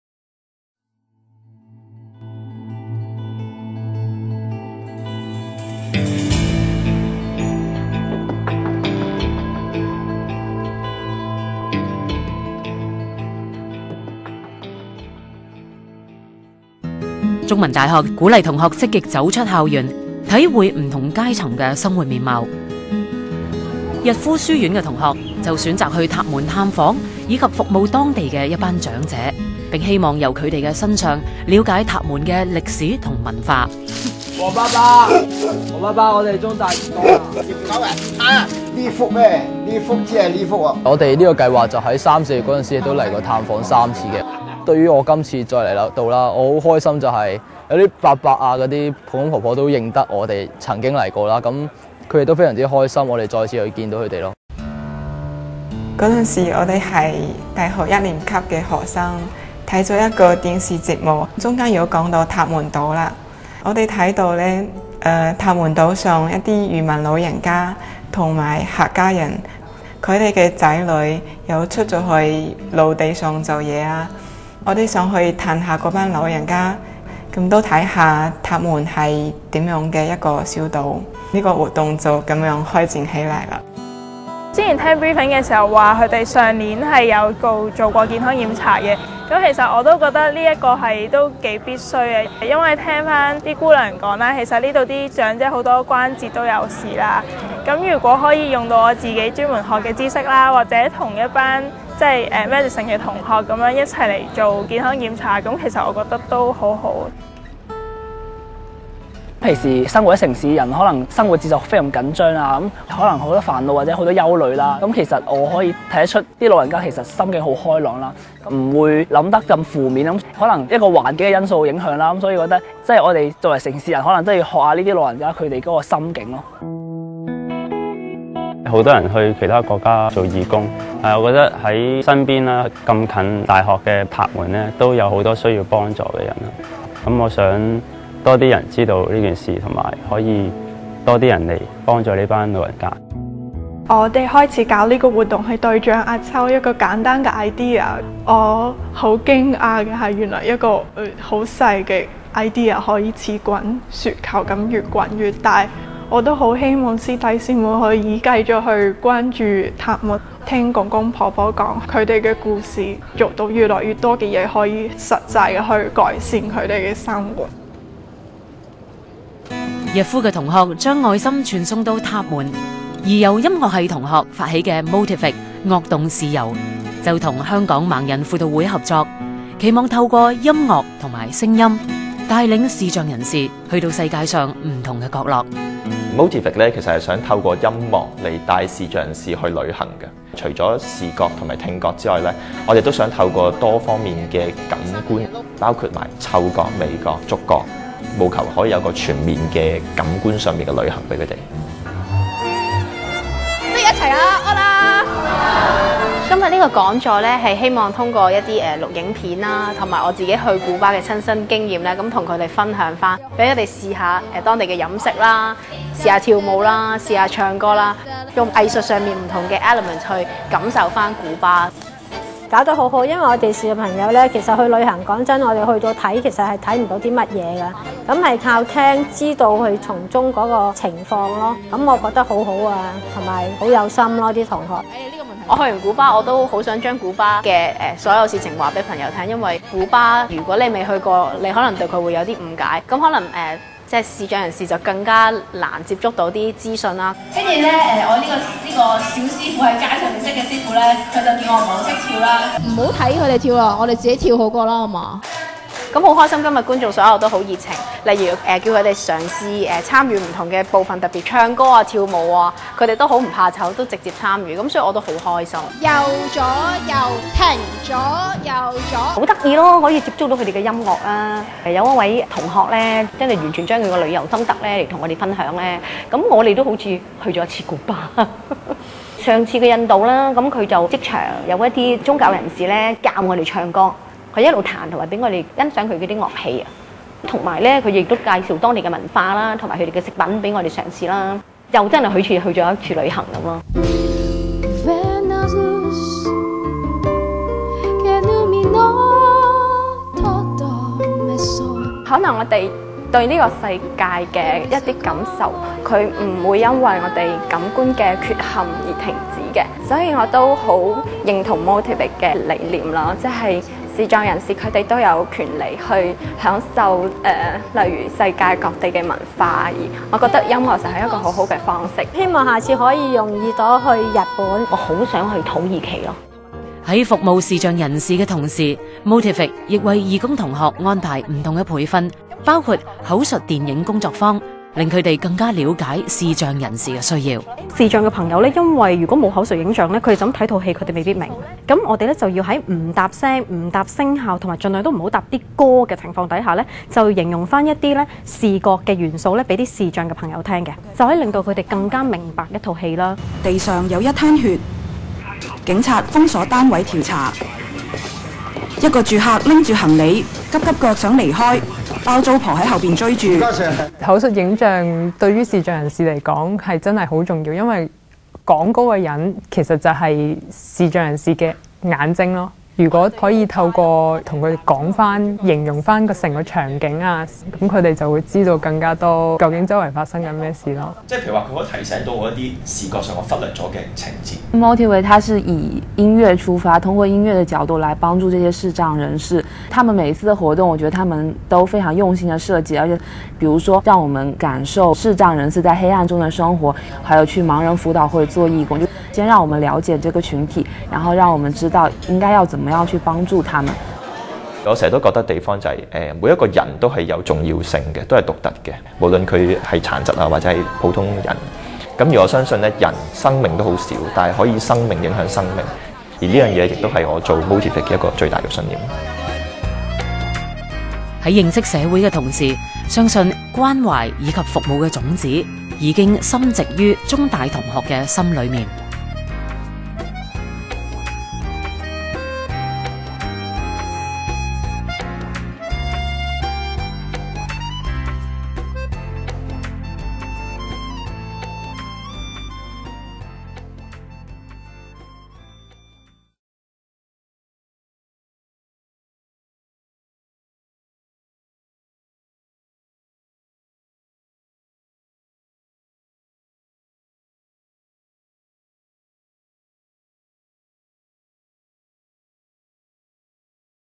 「中大五十．人」走出校園，關懷世界 | "CU50•The People” Stepping Outside the Campus, Showing We Care
中大鼓勵同學走出校園，體會社會不同階層的生活面貌。逸夫書院的同學選擇將關愛帶給塔門島的長者；而音樂系的同學發起「MOTIVIC樂動視遊」，透過音樂讓視障人士感受世界，展現了以生命影響生命的信念。

CUHK has been encouraging students to reach out to the community and experience all walks of life.

The students of Shaw College chose to visit a group of elderly at Tap Mun; while the students from the Music Department initiated the project “MOTIVIC” to help the visually impaired to experience the world through music and sound, believing that lives can influence lives.